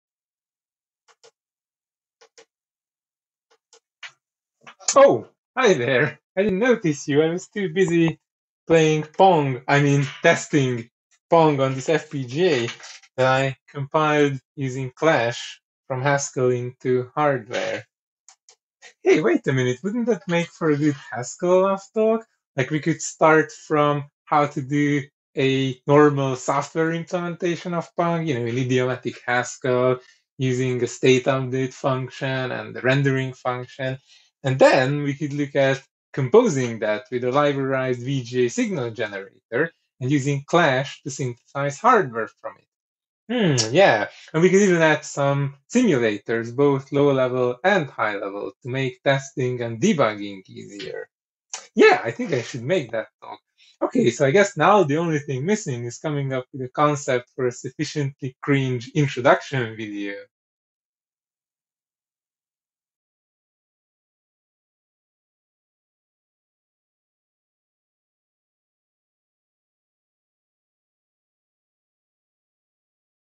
Trailer for "Executable, Synthesizable, Human Readable: Pick Three"
Trailer/intro/ad for my upcoming Haskell Love 2021 talk, which will present the Haskell compiler Clash via the worked example of a simple Pong circuit.